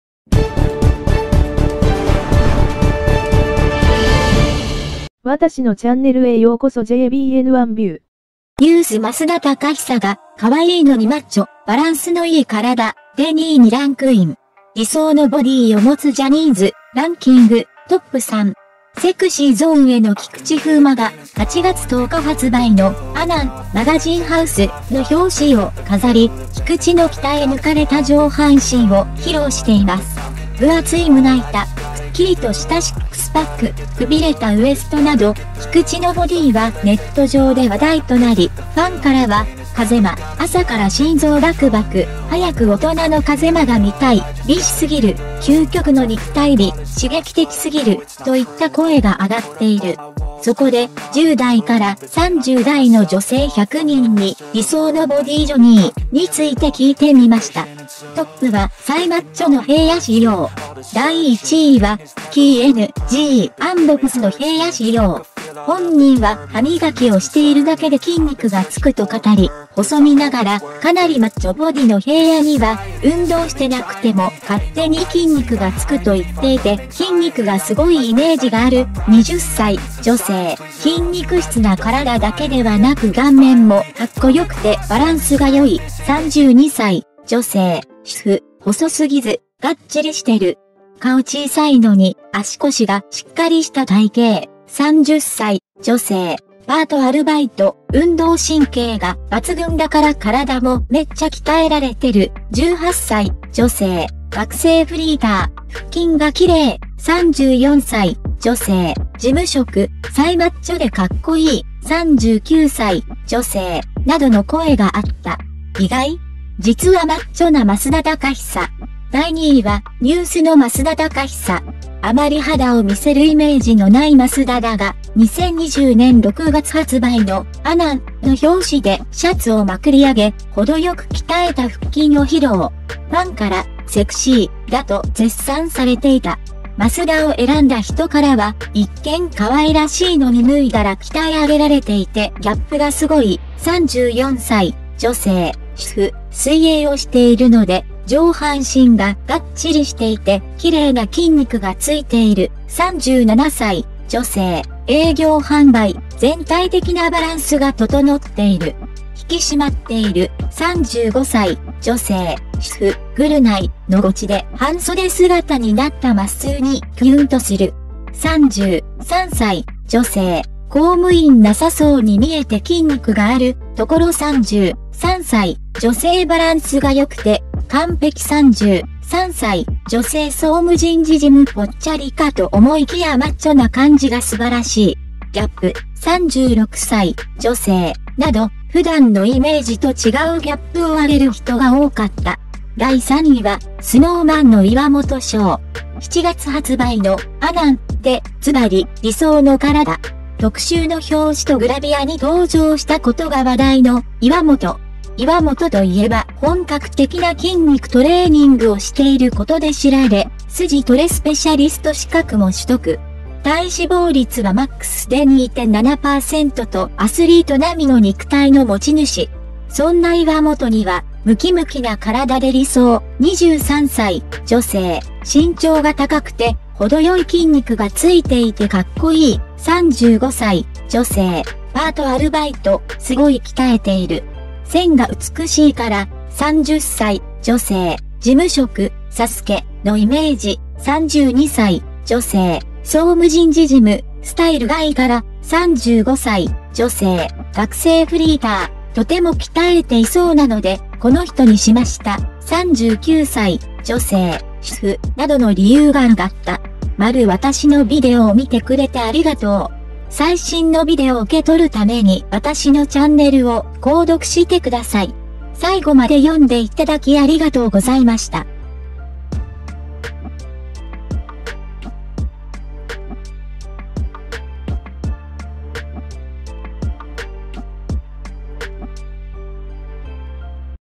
【平野紫耀】意外!？ 実はマッチョな増田貴久。トップは細マッチョの平野紫耀？ 。第１位は？
《動画の内容》
Sexy Zone's Fuma Kikuchi is on the cover of "anan" (Magazine House), which will be released on August 10th, showing off Kikuchi's well-trained upper body. Kikuchi's body, such as a thick chest, a clear six pack, and a constricted waist, became a hot topic on the Internet, and fans commented, "Fuma, my heart is pounding from the morning," "I want to see an adult Fuma soon," and "Too beautiful." ”, ”Ultimate physical beauty”, ”Too stimulating”. So, we asked 100 women in their teens to 30s about their “ideal body Johnny.”